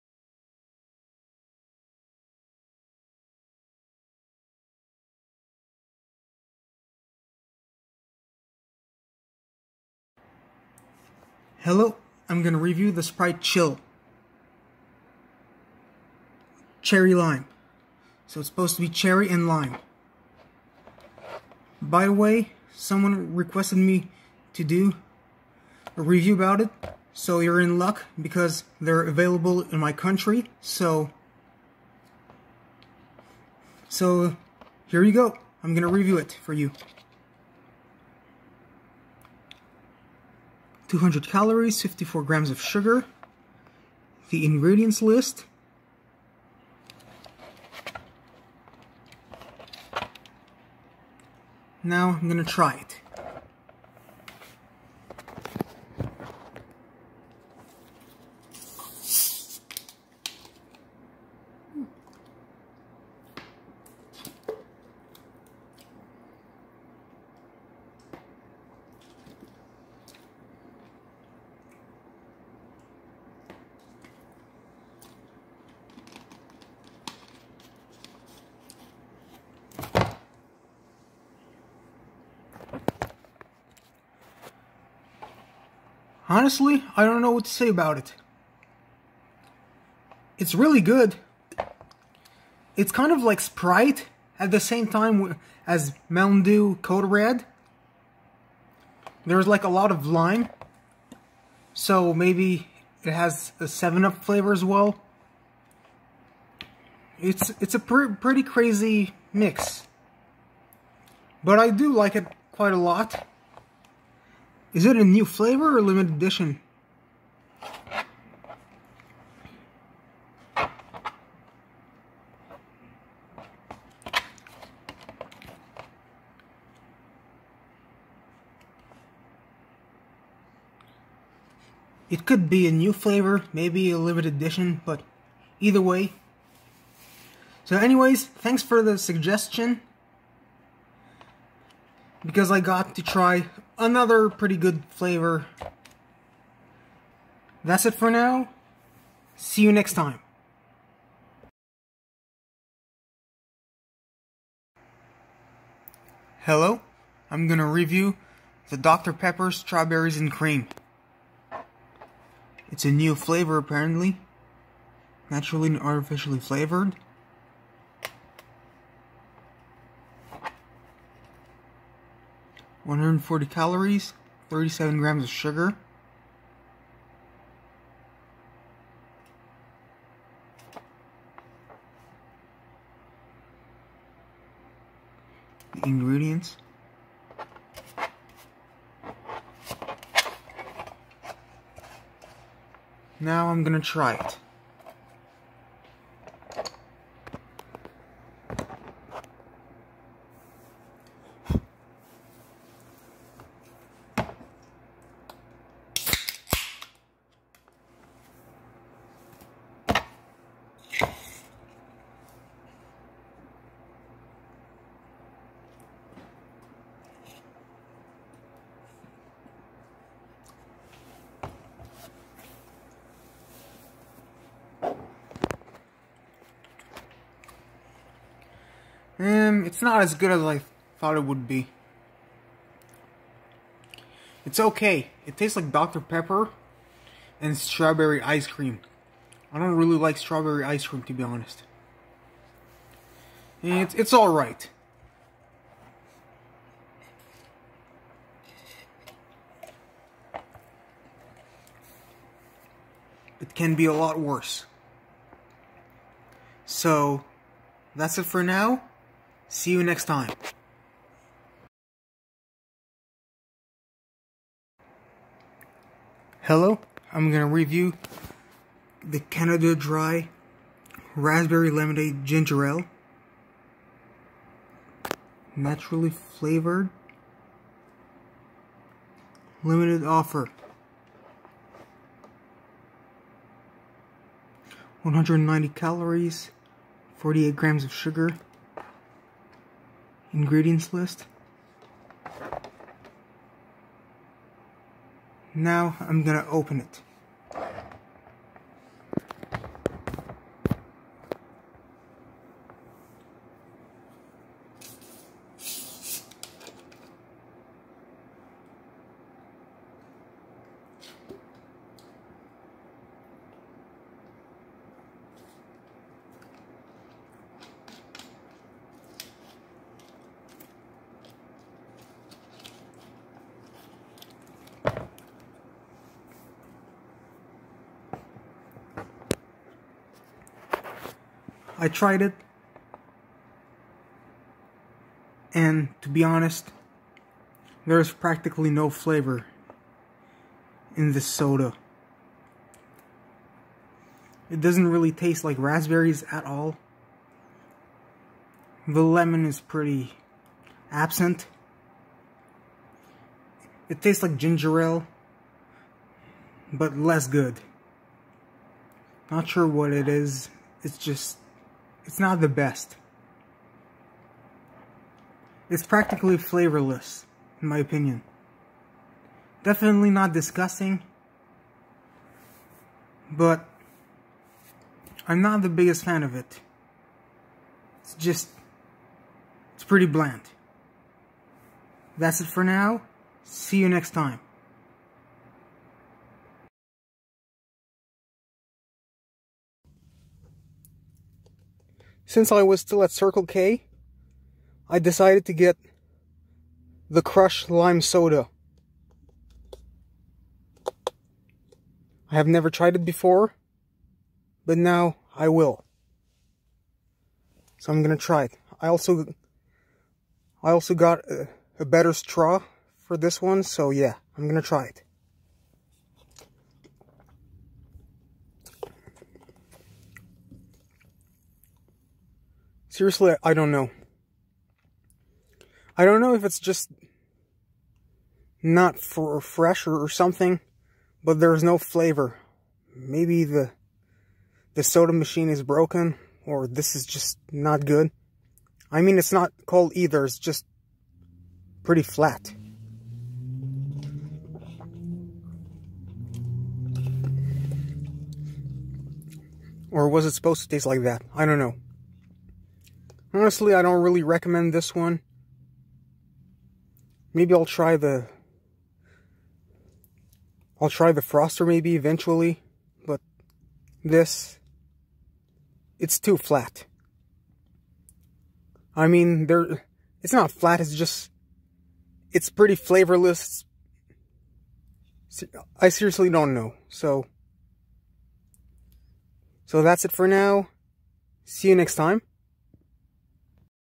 Some soft drinks I reviewed in mid 2024 (compilation)
Here's several soft drink reviews I decided to put into one video. There's Sprite Chill, Dr. Pepper Strawberries & Cream, Canada Dry Raspberry Lemonade Ginger Ale and Lime Crush Soda.

Sprite Chill is a limited edition Sprite flavor. It tastes like a combination of Mountain Dew Code Red, regular Sprite and 7up. I definitely recommend this Sprite flavor. It was also a great suggestion from a YouTube user as I found this flavor extremely good. Dr. Pepper Strawberries & Cream is a new flavor apparently. Not sure if it's a limited edition or not, but it's a new flavor. It tastes like Dr. Pepper and strawberry ice cream. I don't like it that much, but it's still somewhat decent. Canada Dry Raspberry Lemonade Ginger Ale is a limited time only soda. It's probably the blandest soda I've ever tried as there's next to zero flavor in my opinion. It's not offensive, but it tastes like almost nothing. Too bad it doesn't taste like raspberry lemonade that much. The Lime Crush Soda from Circle K might have been faulty since it didn't taste right whatsoever. It barely tastes like anything and it's not sweet like the usual sodas at all. It tasted like carbonated water with very small hints of lime, that's it.

The Sprite Chill review was filmed and originally uploaded on May 7, 2024. The Dr. Pepper Strawberries & Cream was filmed and originally uploaded on May 31, 2024. The Canada Dry Raspberry Lemonade Ginger Ale review was filmed and originally uploaded on June 11, 2024. The Lime Crush Soda was filmed and originally uploaded on July 13, 2024.